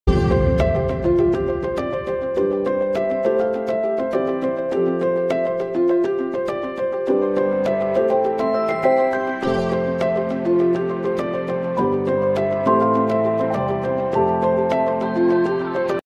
channel update
helo guys Iam here to tell u 1 update.
we are planning to start mincraft survival series.If u want that video plz comment

Musician: @iksonmusic